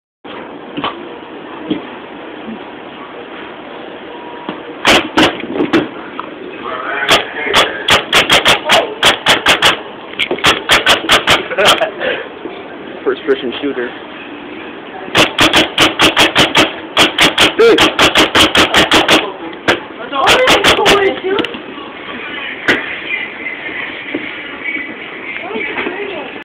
1st person painting
fucking aorund with paint ball gunz